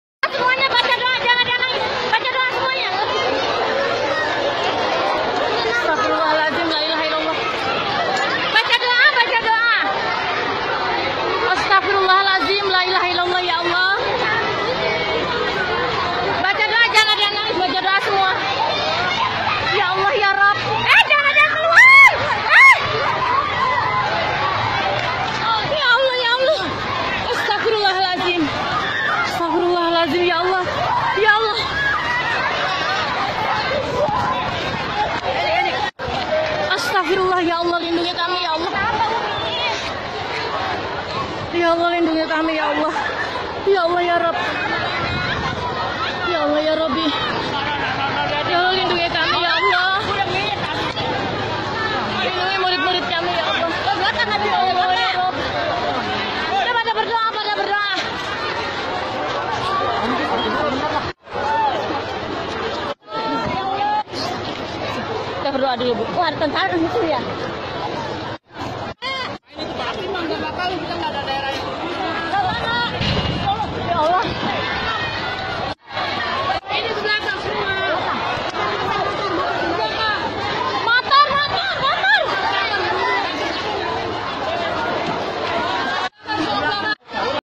Pabrik Petasan Meledak di Kosambi Tangerang Banten
Kamis 26/10/2017-Pabrik petasan di Kosambi Tangerang Banten meledak & terbakar... 16 orang korban meninggal dan puluhan luka-luka.